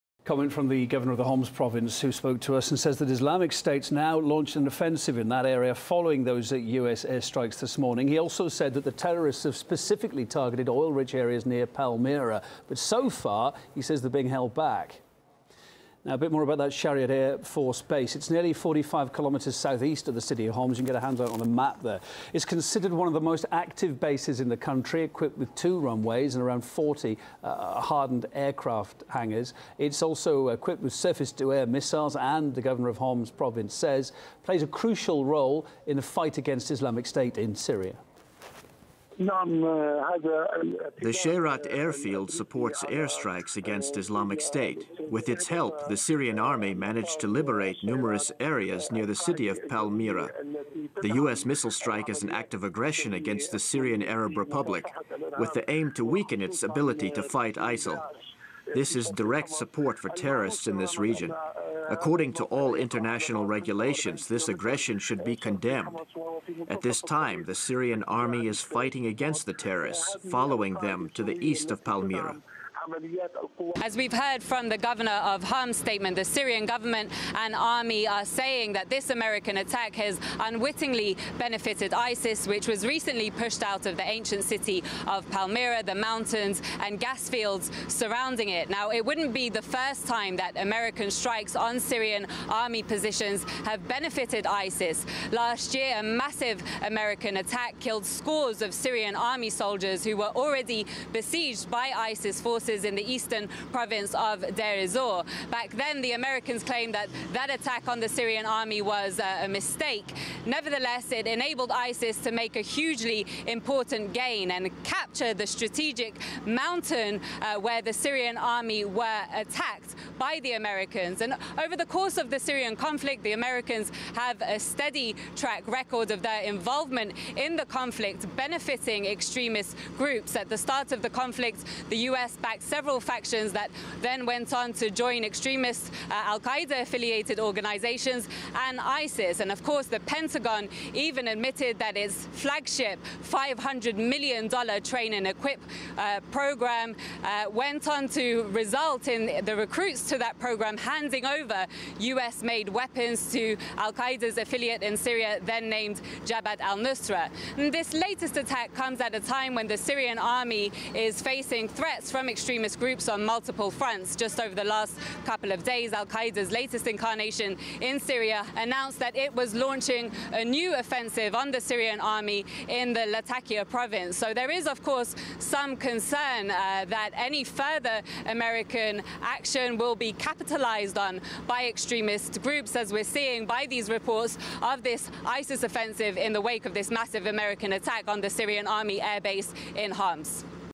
ISIS Used U.S. Missile Strike To Launch New Offensive Near Palmyra - Homs Governor
April 7, 2017 - Taking advantage of the US missile strike on a Syrian airbase, Islamic State forces launched a major offensive to gain control of strategic oil areas near Palmyra, but failed, a local governor told RT.